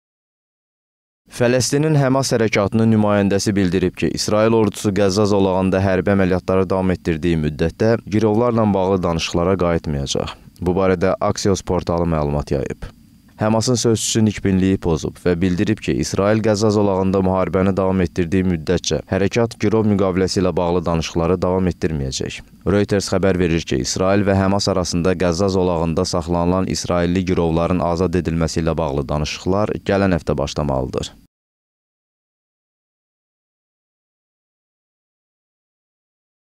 HƏMAS İsraillə danışıqlardan imtina etdi – APA TV
Fələstinin HƏMAS hərəkatının nümayəndəsi bildirib ki, İsrail ordusu Qəzza zolağında hərbi əməliyyatları davam etdirdiyi müddətdə girovlarla bağlı danışıqlara qayıtmayacaq. Bu barədə “Axios” portalı məlumat yayıb.
"HƏMAS-ın sözçüsü nikbinliyi pozub və bildirib ki, İsrail Qəzza zolağında müharibəni davam etdirdiyi müddətcə hərəkat girov müqaviləsi ilə bağlı danışıqları davam etdirməyəcək.
“Reuters” xəbər verib ki, İsrail və HƏMAS arasında Qəzza zolağında saxlanılan israilli girovlarının azad edilməsi ilə bağlı danışıqlar gələn həftə başlamalıdır.